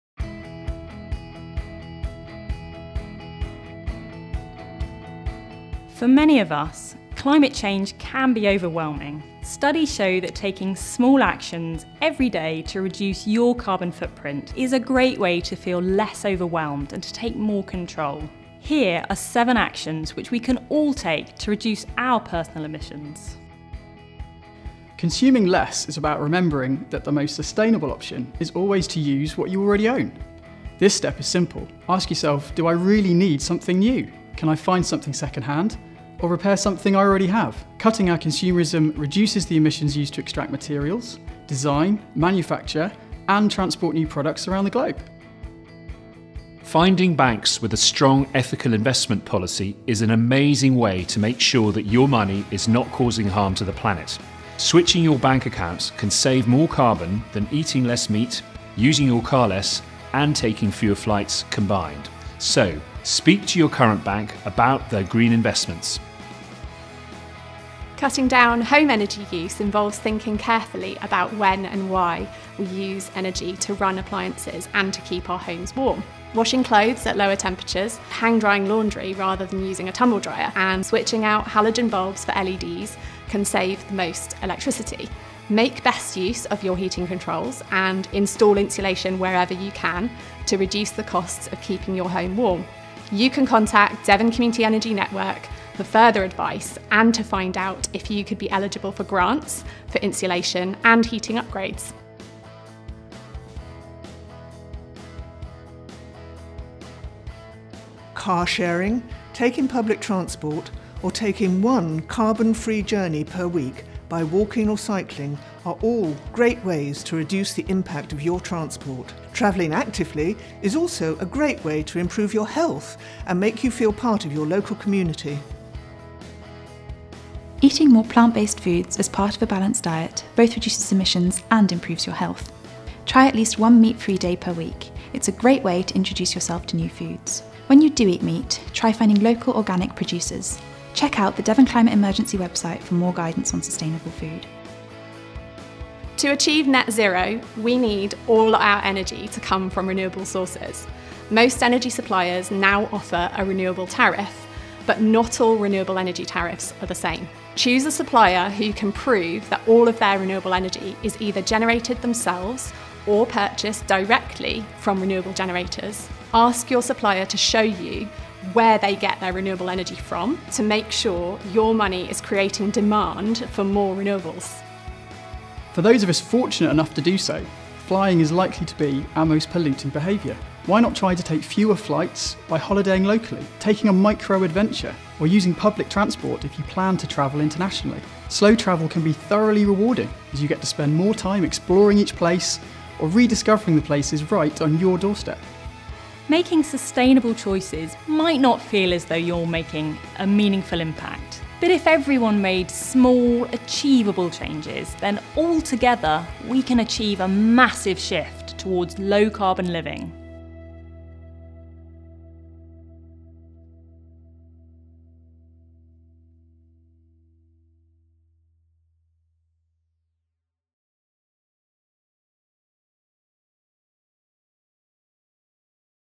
Seven tips to reduce your carbon emissions
The tips in this video are easy to achieve, and will help you reduce your personal carbon emissions.